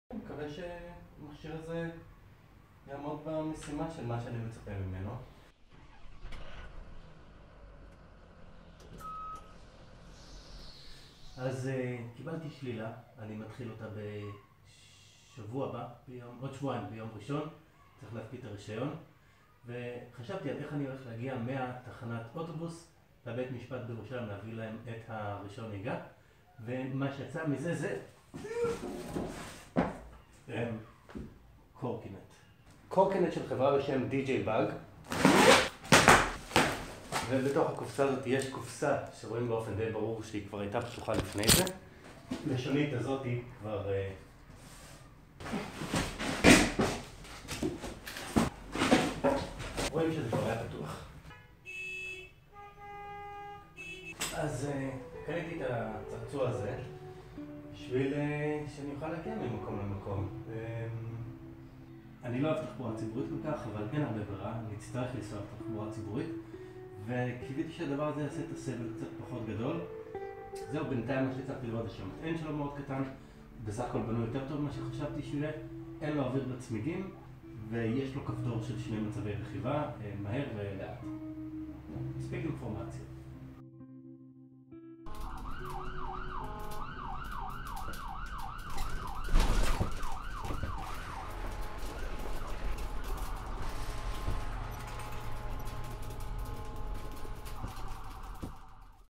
קורקינט חשמלי, ב1000 שקל
ניסוי בבן אדם למשך 8 חודשים ללא רכב, הזמנתי משלוח מהיר של הקורקינט הזול השוק ה JDBUG קורקינט ב 1000 שח,
תחילת
gh4 ממש לא אוהב לעשות פוקוס
סאונד גרוע

YOUTUBE שינו את החוזה ליוצרים